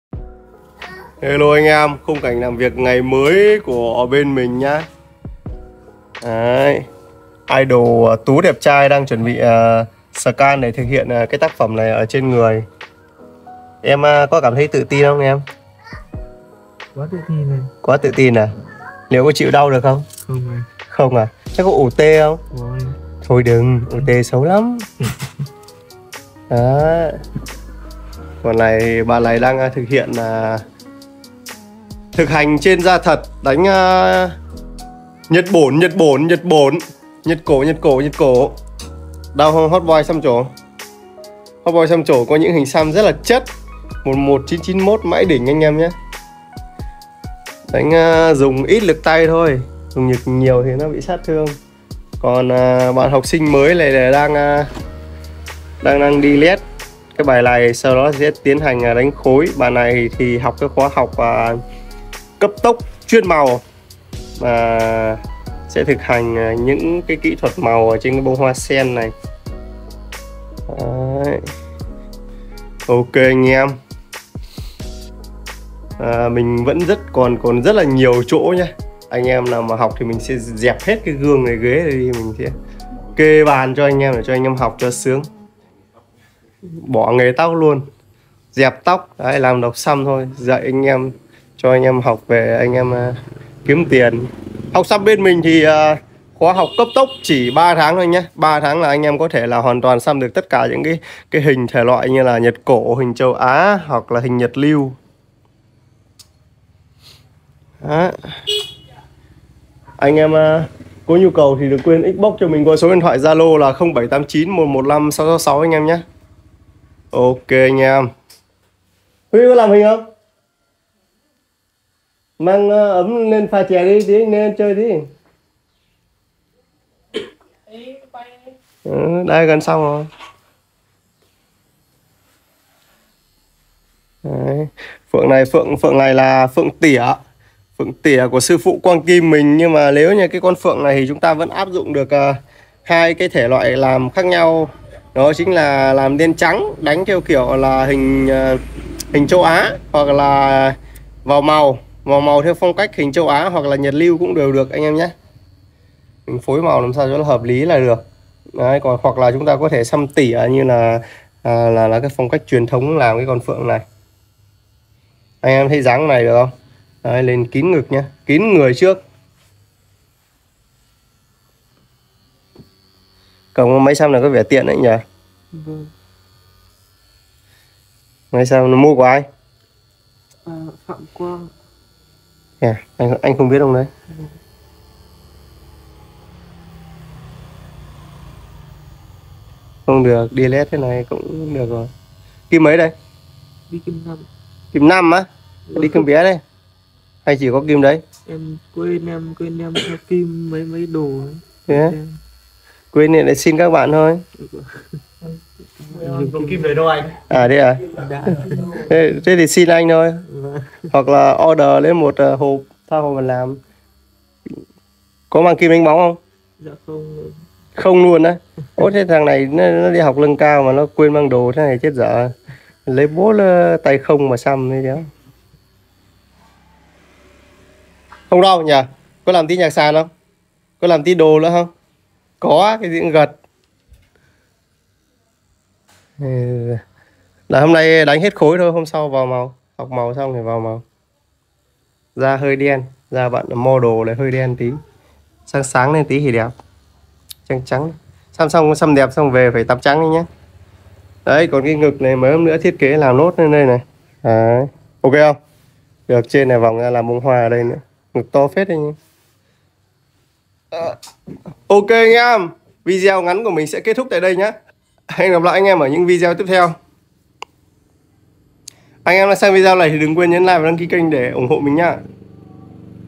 Dậy xăm cấp tốc, 1 ngày bận rộn của Tâm sáng tattoo
_ Dậy xăm cấp tốc, dậy xăm online kèm 1/1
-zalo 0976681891